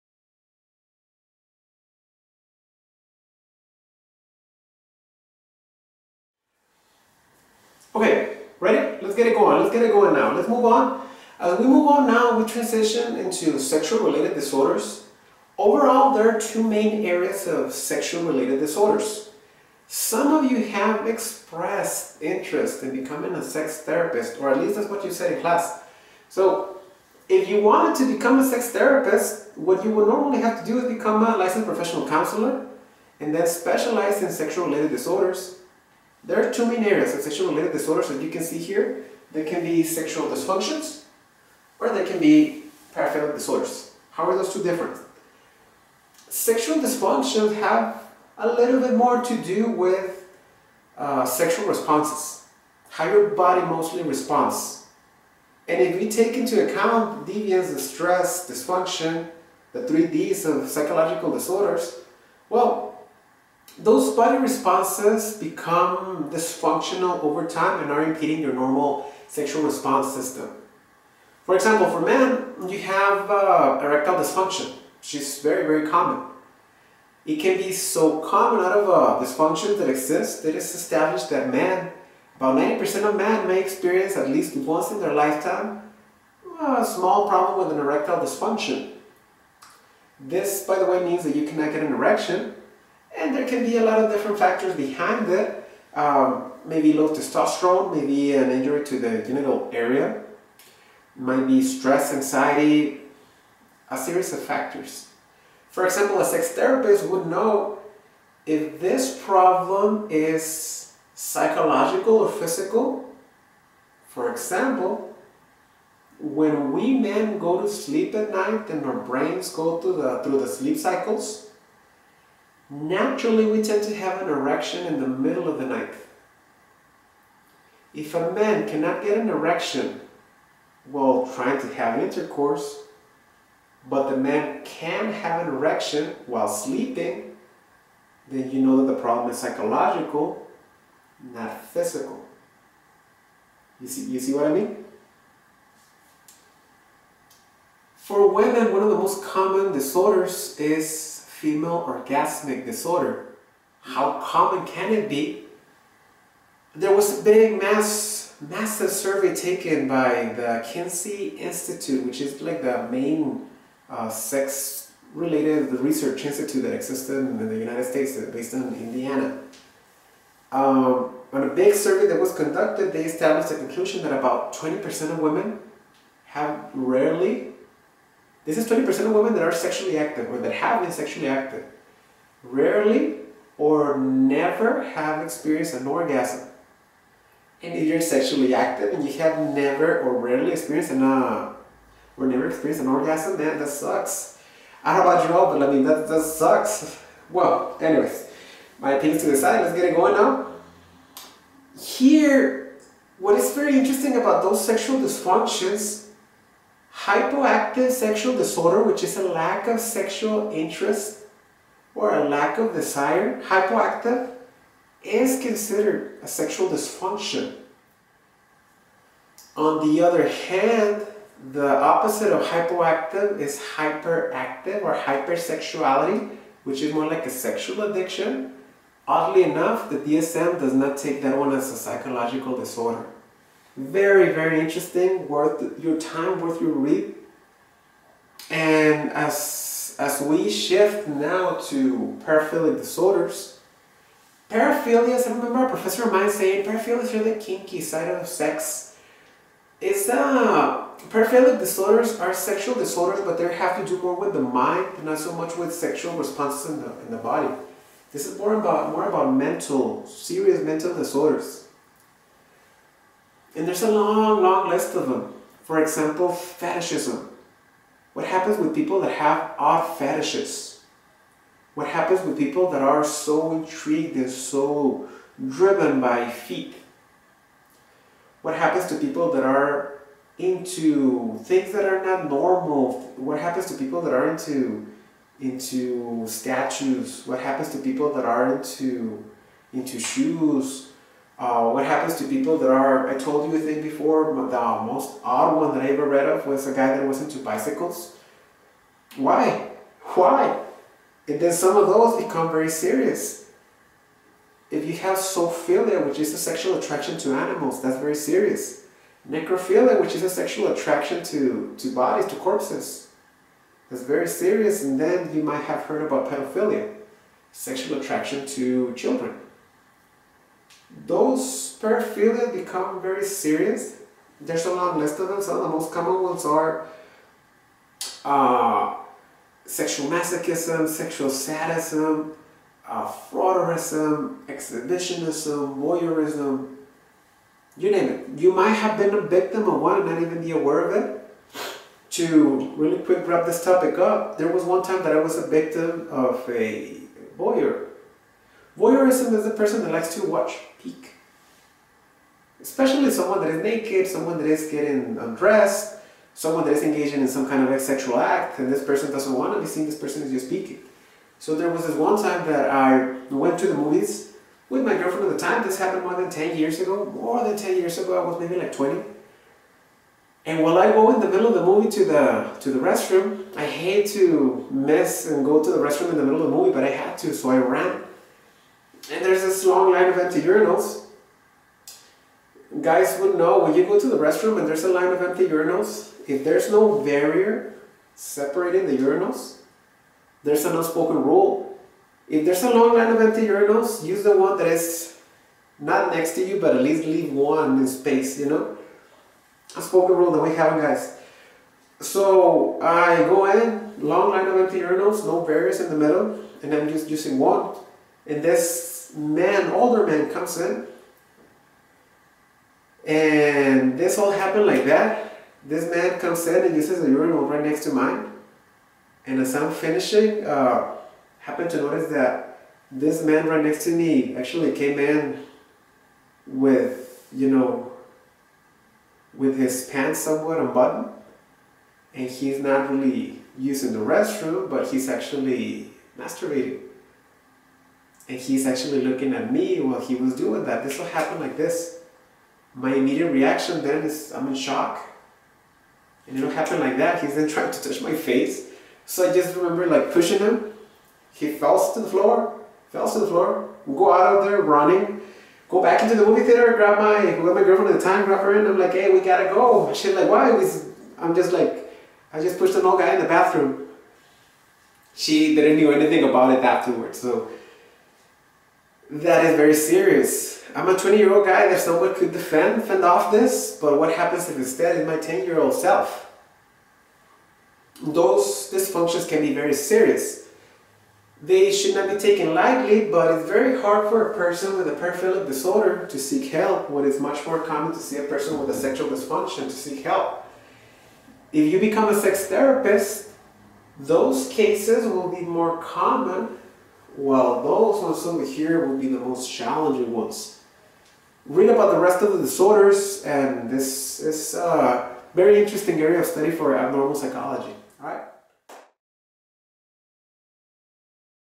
Abnormal Psyc - Sexual disorders
This video will teach you the main difference among sexual dysfunctions and paraphilic disorders